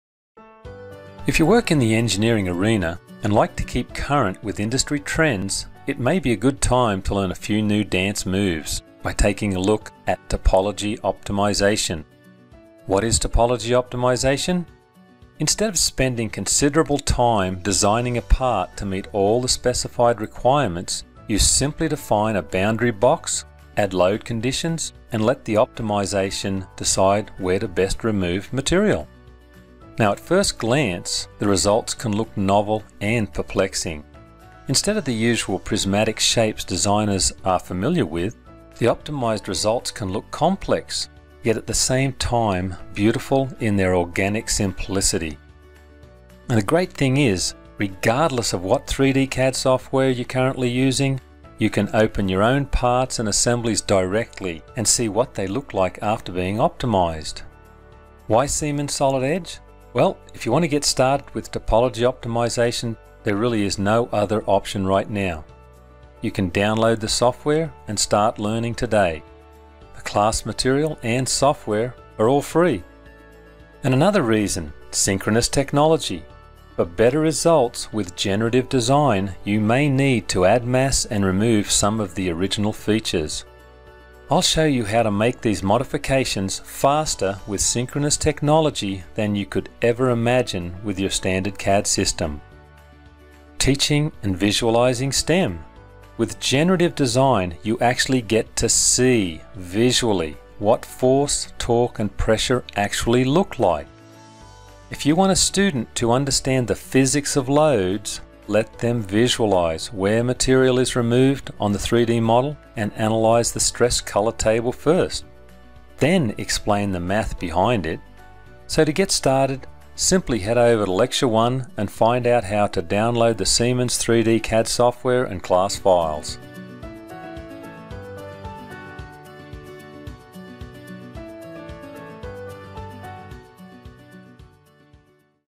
Generative Modeling Structural Optimization- Siemens Solid Edge Tutorial
In this demonstration, we show you how Solid Edge continues to deliver innovation in product development with built-in Generative Design capabilities. Generative design produces an organic, reduced-mass geometric solution of a specific material optimized within a defined space, accounting for permissible loads and constraints.